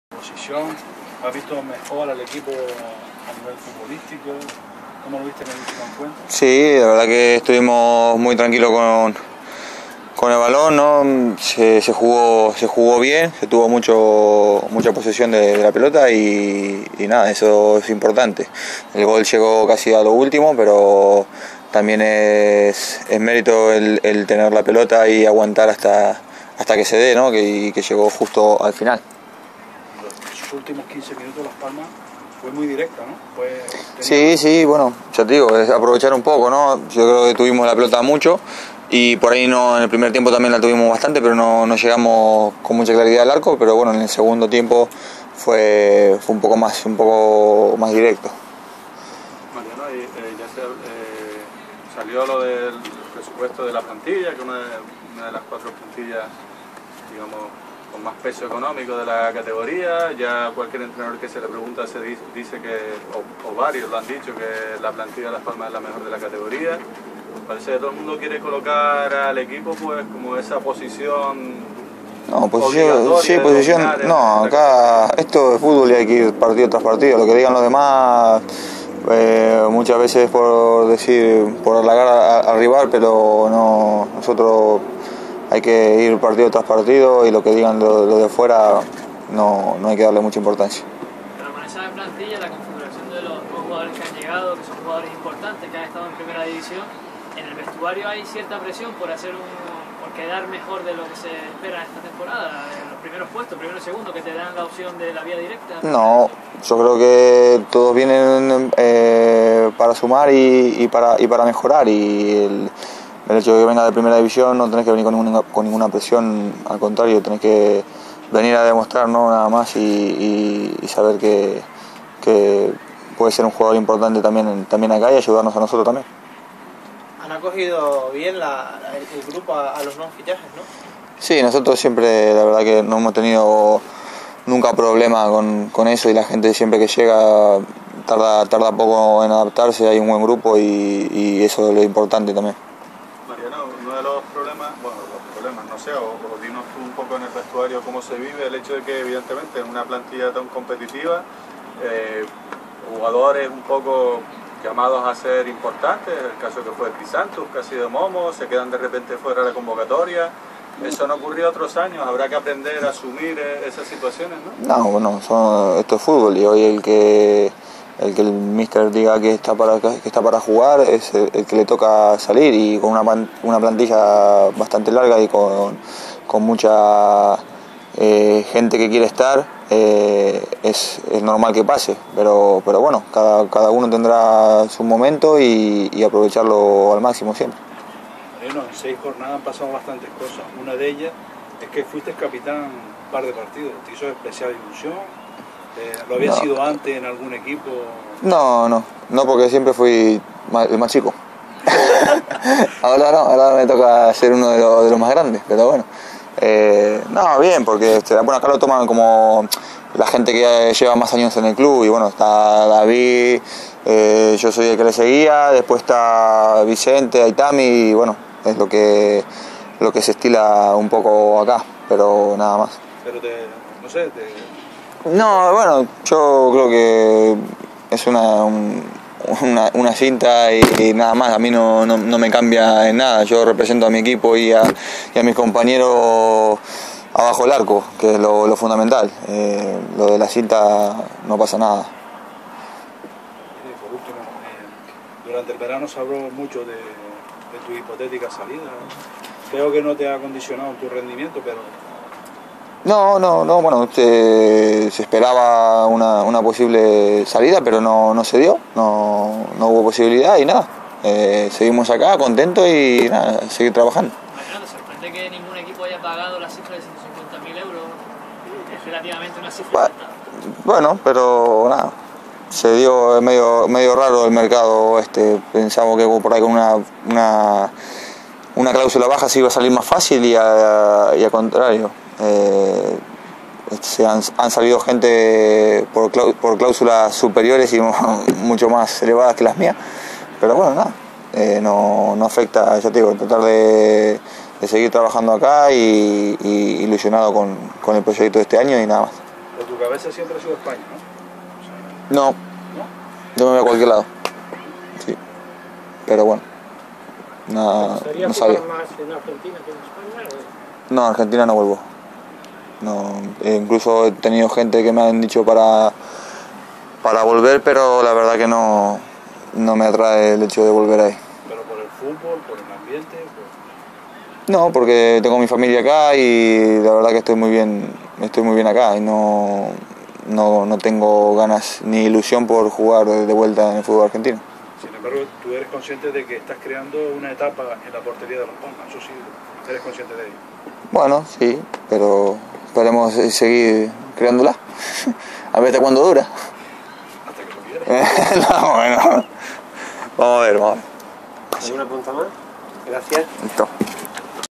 Mariano Barbosa, arquero de la UD Las Palmas en rueda de prensa.
Caseta de prensa de Barranco Seco, martes 24 de Septiembre 2013.

El guardameta de la UD Las Palmas, comparece ante los medios de prensa a la finalizción del entrenanito de la tarde.